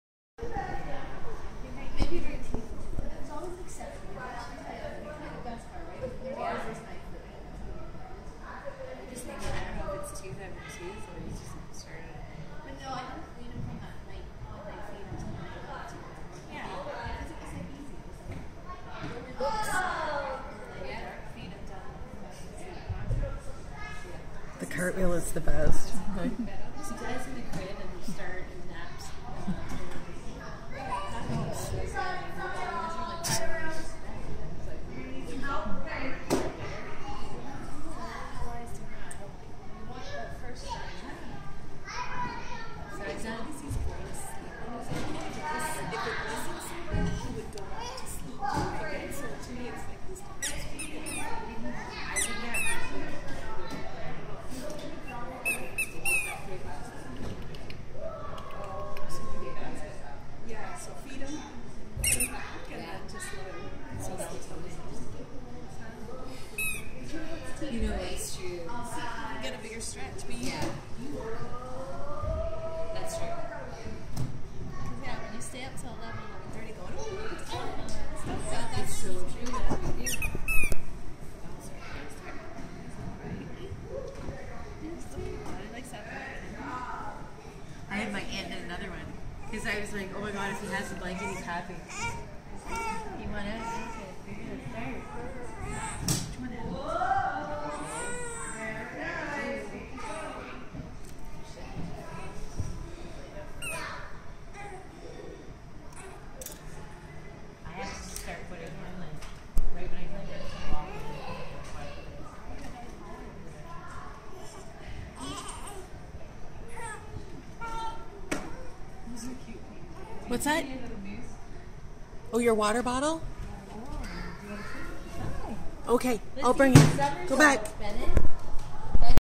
Headband and legwarmers!
Another week at Gymnastics,  this week Dannee's sporting her very shiny bodysuit, pink headband and black legwarmers!!  Wait for the end of video!! Lol